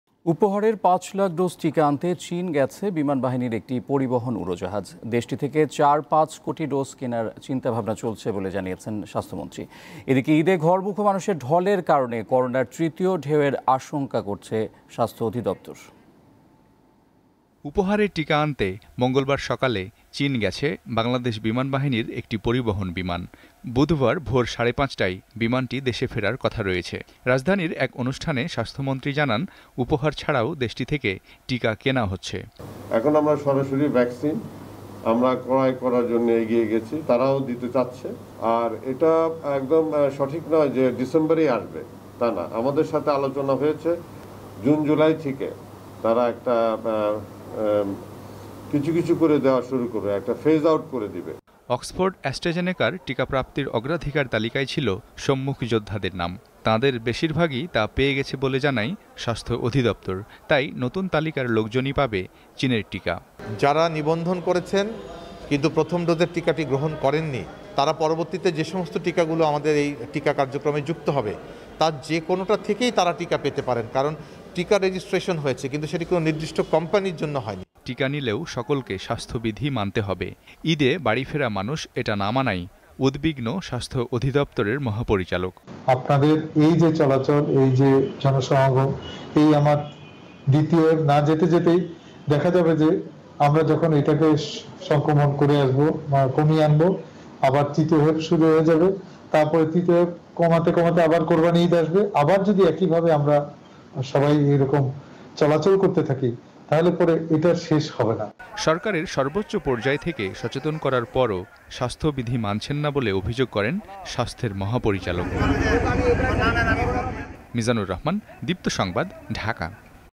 টিকা আনতে চীনে বিমানবাহিনীর উড়োজাহাজ|Air force Flight In China| Deepto News| Deepto TV
উপহারের পাঁচ লাখ ডোজ টিকা আনতে চীন গেছে বিমান বাহিনীর একটি পরিবহন উড়োজাহাজ। দেশটি থেকে চার-পাঁচ কোটি ডোজ কেনার চিন্তা-ভাবনা চলছে বলে জানিয়েছেন স্বাস্থ্যমন্ত্রী। এদিকে, ঈদে ঘরমোখো মানুষের ঢলের কারণে, করোনার তৃতীয় ঢেউয়ের আশঙ্কা করছে স্বাস্থ্য অধিদপ্তর।

Deepto TV address:
7/A/GA Tejgaon Industrial Area, Dhaka 1208, BANGLADESH